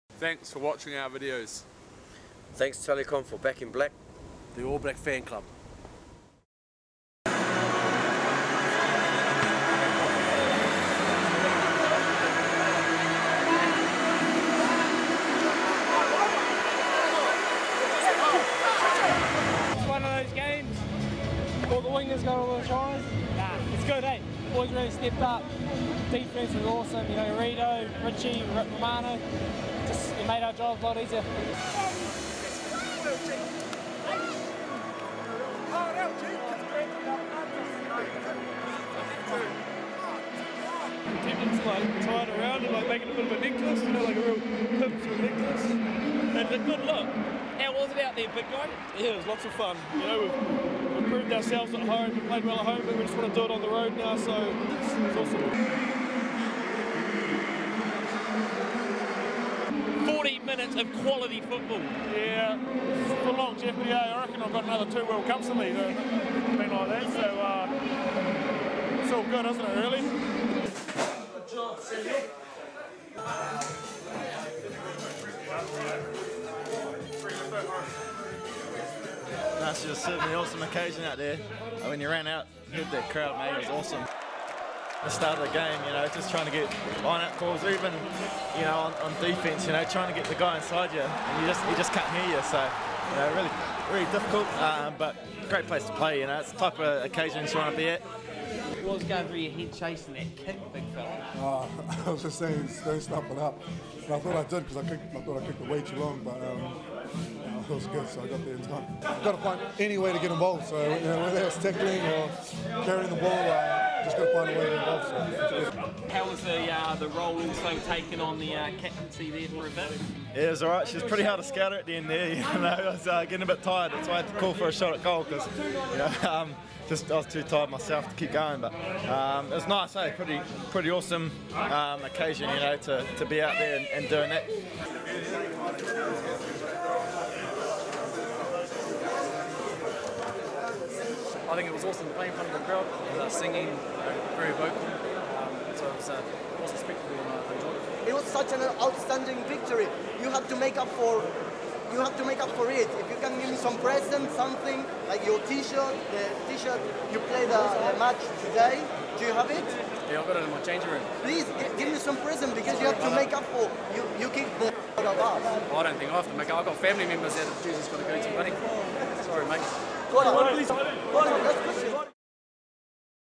All Blacks Win Rugby Championship in Argentina
Hear what the All Blacks have to say after their game against Argentina. Head into the All Blacks changing room and share in their achievement.